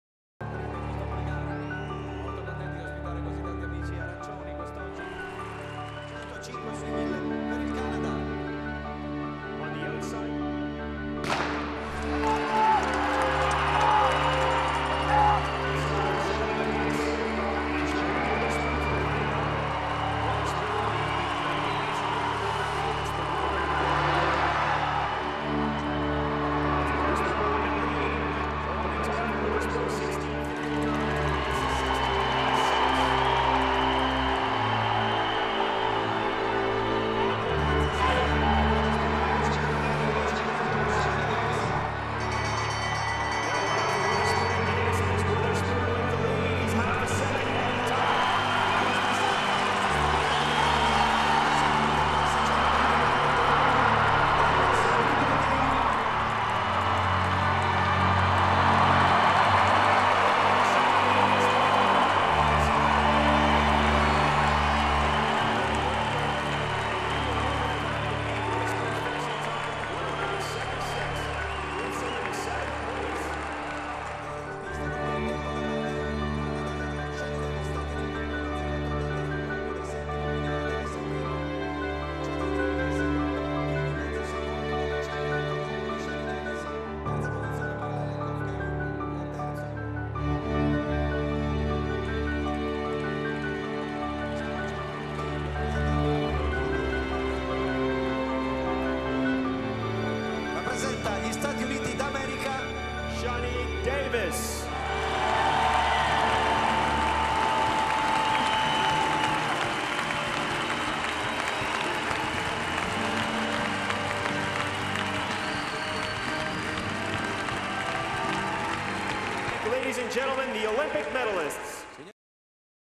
Davis - Speed Skating - Men's 1000M - Turin 2006 Winter Olympic Games
Davis flies to victory in the men's 1000M speed skating event at the Turin 2006 Winter Olympic Games.

Athletes featured in this video :
DAVIS Shani, United States of America :